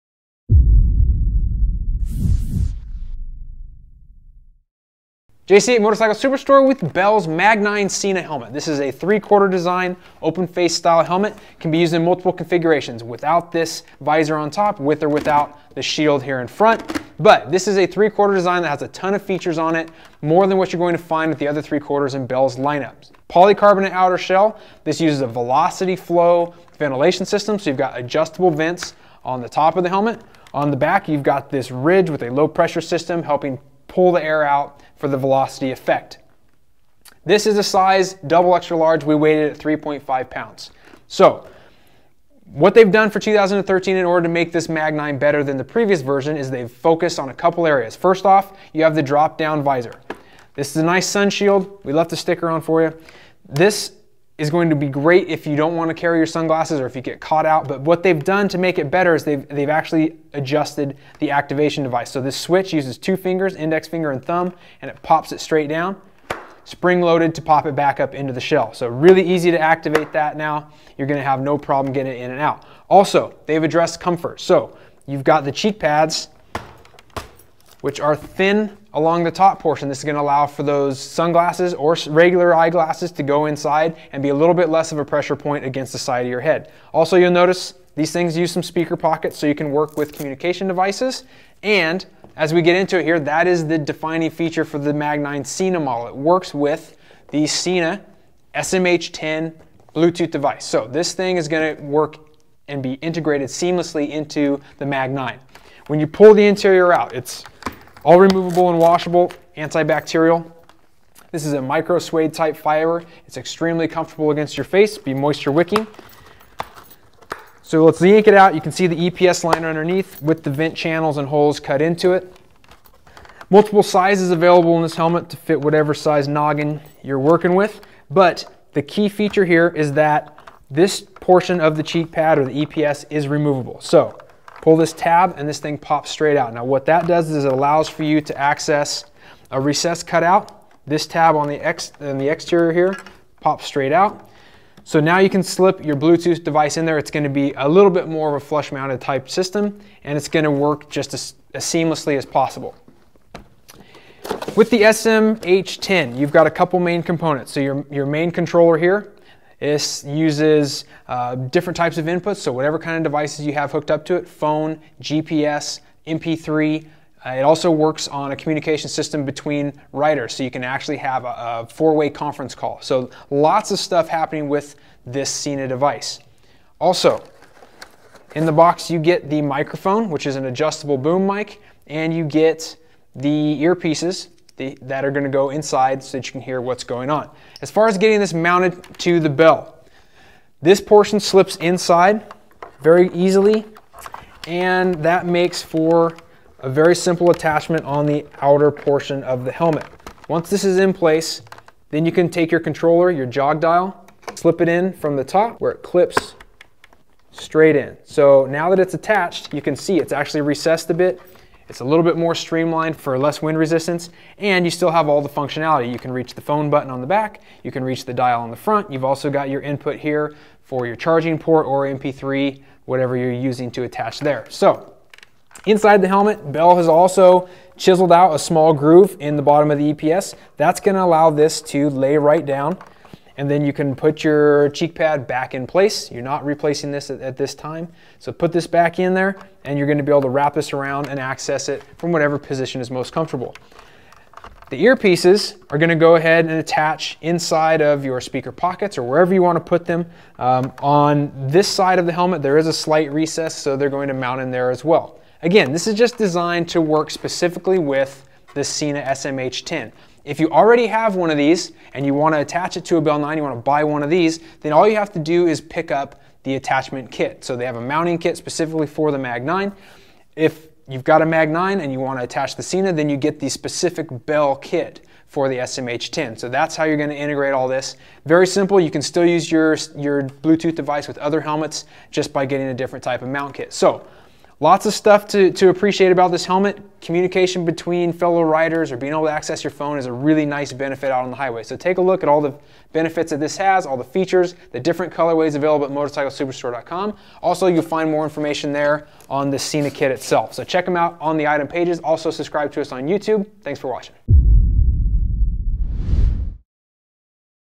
Bell Mag-9 Sena Helmet from Motorcycle-Superstore.com
A good 3/4 helmet provides the protection and freedom that no other style can match. The Bell Mag-9 Sena brings more to the table than other open-face designs with the seamless integration of Bluetooth technology from Sena Technologies. Dedicated modifications to the polycarbonate shell and interior liners provide access to the Sena SMH-10 headset so riders can access their cell phones, MP3 music devices or GPS while operating their street bike. Other riders using Sena technology are able to communicate directly with a conference call feature. Never miss that important turn, fuel stop or bathroom break. Staying in touch with riding companions makes your next ride that much easier to coordinate and more enjoyable. Ultra comfortable and versatile, this Bell motorcycle helmet has unmatched features and quality. It's a great match for long distance touring or cruising your favorite local roads. It even works great for UTV and Side-by-Side driving. The Mag-9 is the one helmet that does it all in style.

Bell MAG-9 Sena Helmet Features:
Tough and lightweight polycarbonate shell
3 EPS sizes for maximum comfort and size range
Helmet can be run in multiple configurations: with or without shield and with or without visor
Fast and easy tool-free shield changes
Drop-down sunshield offers "on the fly" protection from the sun
Velocity Flow™ ventilation creates the most ventilated helmet in the open face category
Convenient speaker pockets make it easy to set up the Mag-9 with communication devices
Removable and washable interior for maximum comfort
Contour cheekpads for superior fit and comfort
DOT certified
Communications sold separately